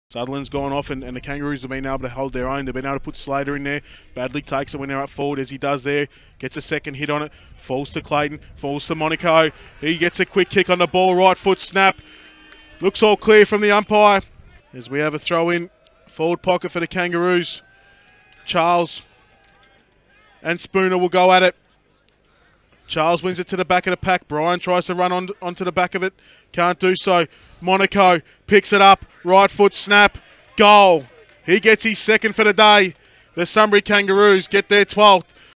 Christian Monaco Returns In Style
Playing in his first senior game of the year, Sunbury Kangaroo Christian Monaco slotted a couple of nice snags in Saturday's win over the Northern Saints.